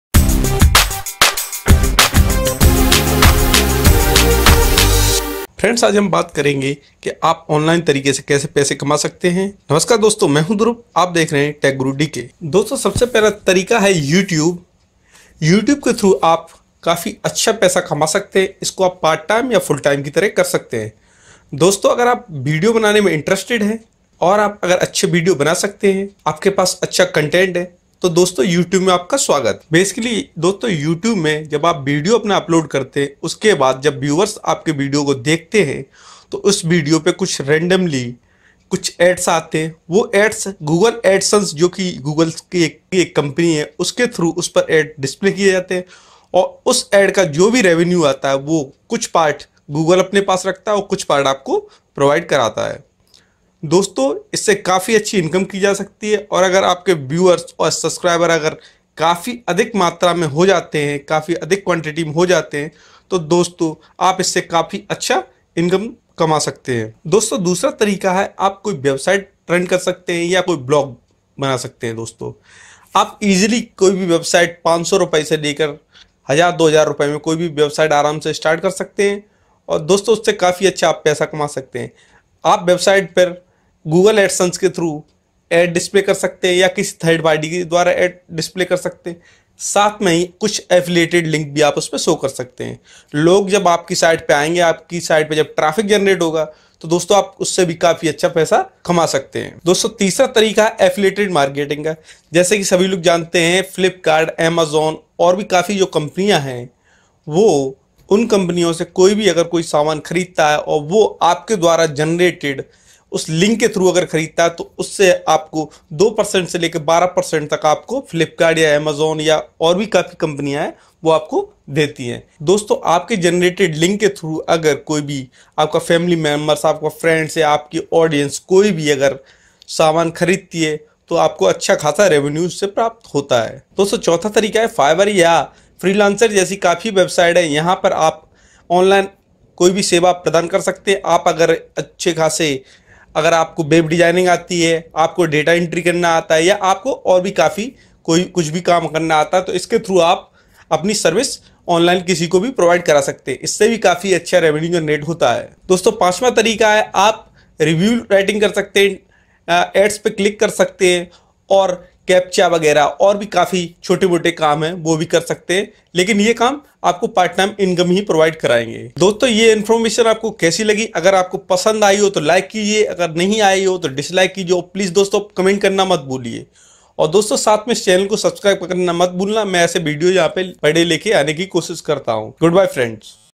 How to earn money online from internet |2017|India |Hindi
We are talking about “how to earn online money ”. There are few ideas, which we are following.
1: - Youtube - you can earn money by youtube, where you upload videos and Google Adsense providing ads on it and you earn some good money from that.
2: - Website, Blogs- you cam make website or blog and show some ads on it and by these ads you can earn money.
3: - Affiliated marketing- By affiliate marketing, you can earn good money from some online shopping website like Flipkart and Amazon. you generate some like and some buddy stopped by that then you earn some percent of money by that.
4: - Online service providing- like the fiver, freelancer. you can provide some services like web designing, data entry etc.and earn money.
5: - Review writing, ads clicking, captcha filling these part time type of works you can do and earn some money.

I hope you like this information:-)

Follow on:-